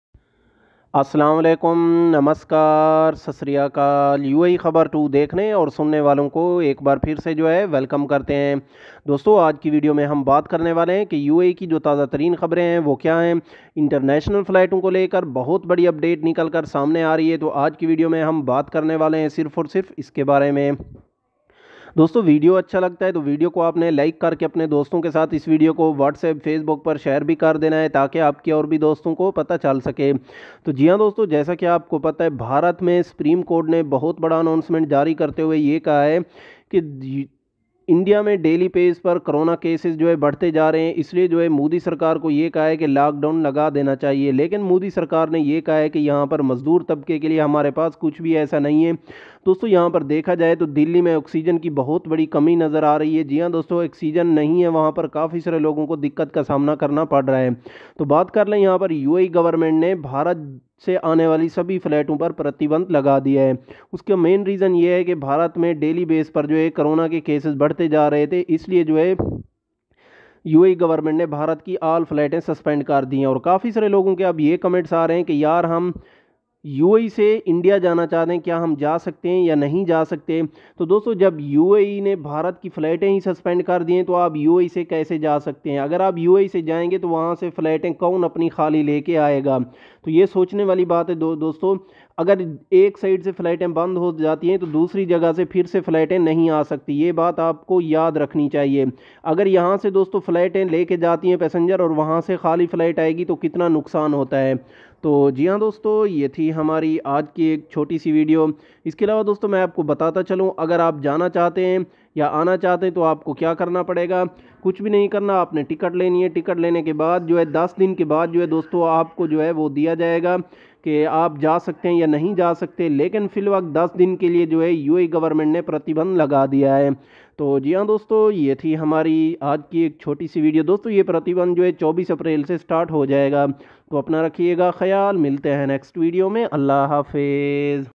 यूएई ने 24 अप्रैल से भारत जाने और आने वाली फ्लैट्स पर प्रतिबंध लगा दिया || UAE indian Flight Ban
भारत से दुबई जाने वाली सभी फ्लाइट 24 अप्रैल से बंद हो जाएगा, India To Dubai Flight Banned 🚫

India se Dubai ke liye 24 April Se 10 Days ke liye All Flight Banned 🚫 Ho jayega
Dubai Jane ke liye other Countries Me 14 Days Stay Hone ke bad Dubai Ja sakte hai